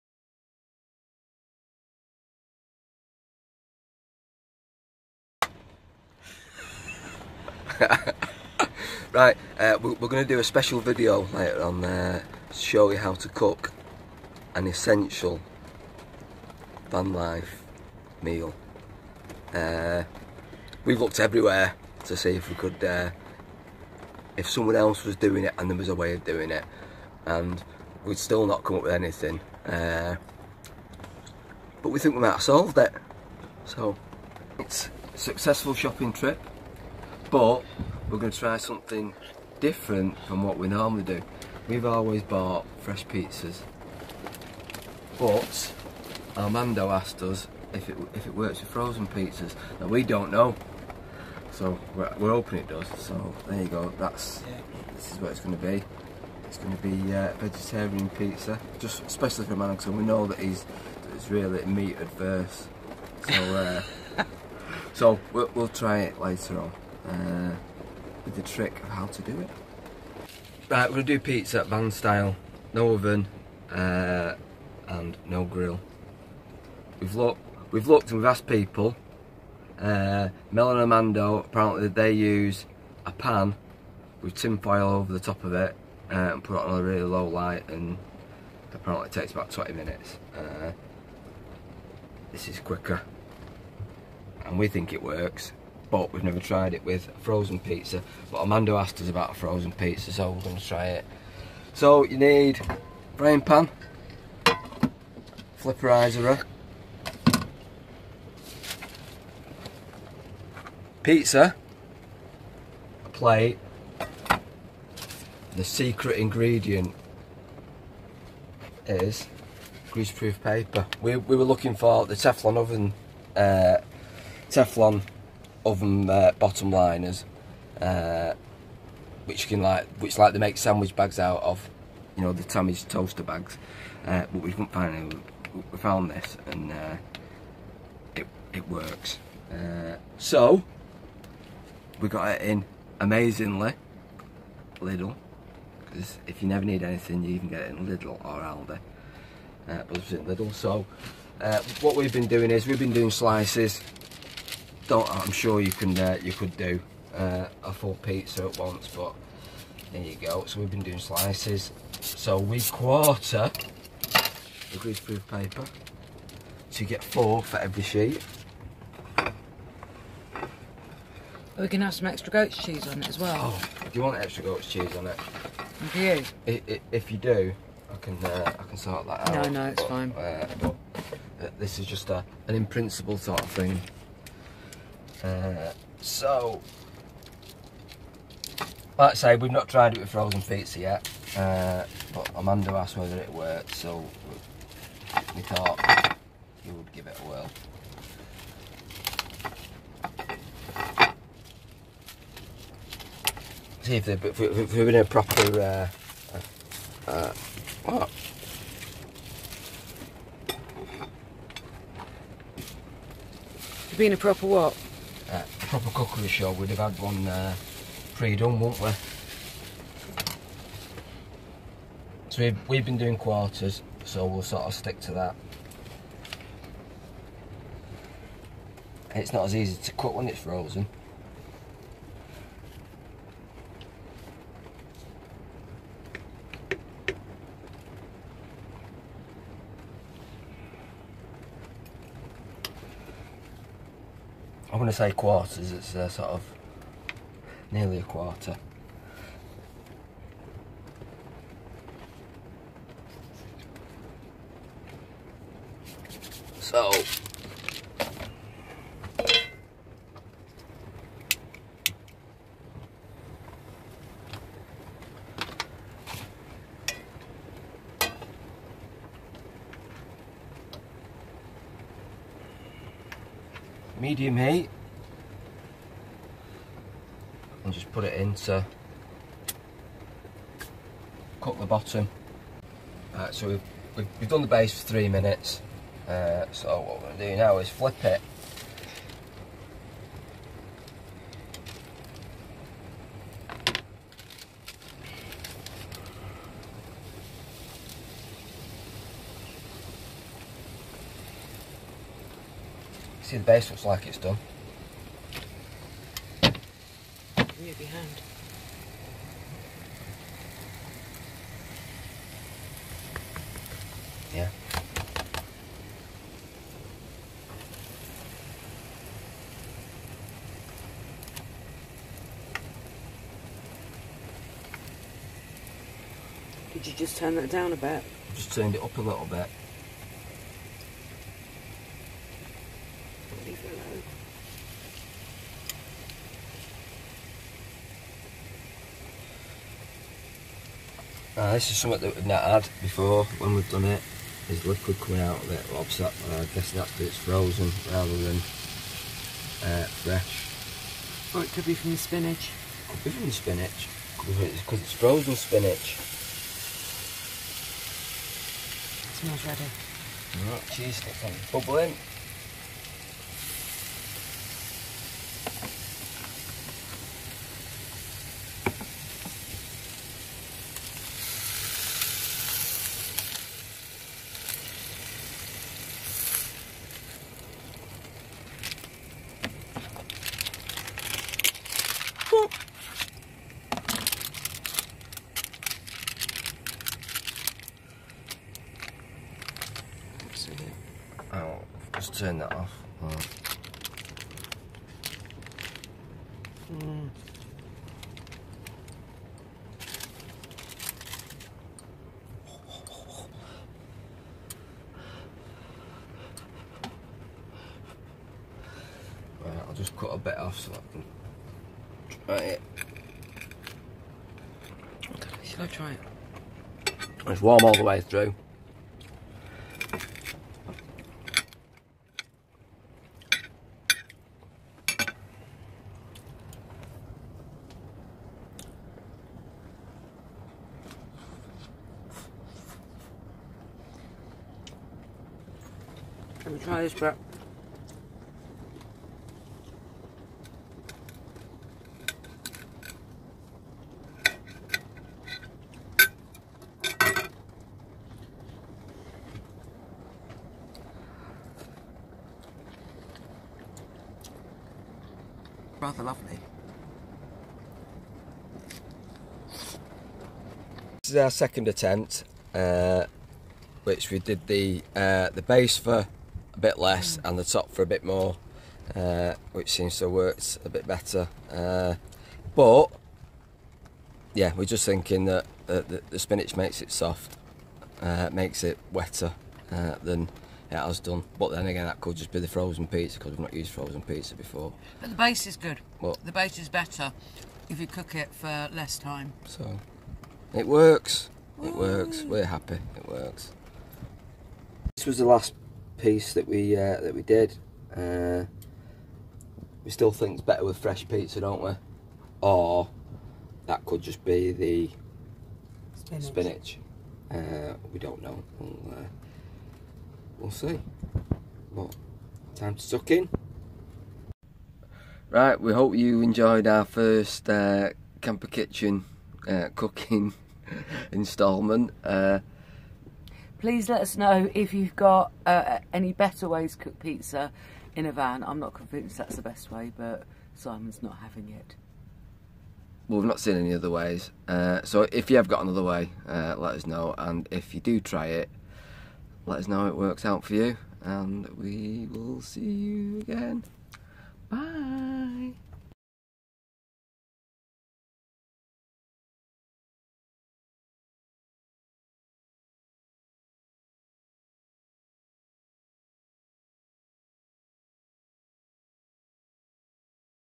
Ultimate Vanlife Pizza Cooking
First world vanlife problems, how do you cook a pizza in a van with no oven or grill? Well we've got one solution.